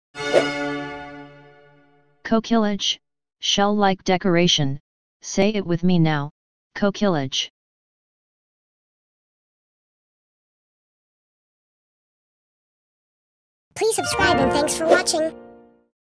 coquillage - Smart & Obscure English Words Defined 🗣🔊
This video is about "coquillage". Here's a video list of some of the most obscure english words with definitions that you might use as a curiosity in your conversations.